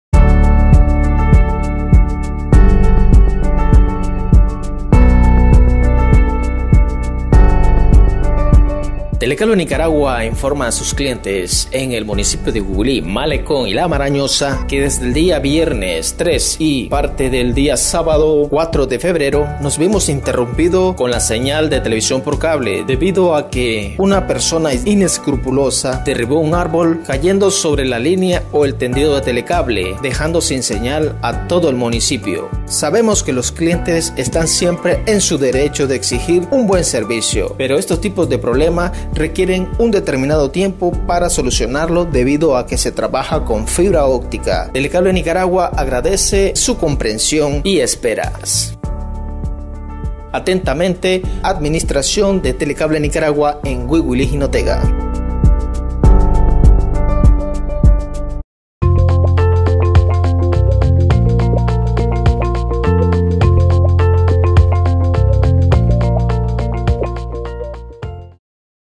corte telecable
TELECABLE Nicaragua reporta que les han estado cortando la fibra óptica por personas inescrupulosas para crear malestares en sus clientes. Ya van más de 12 cortes hasta la fecha de manera intencional.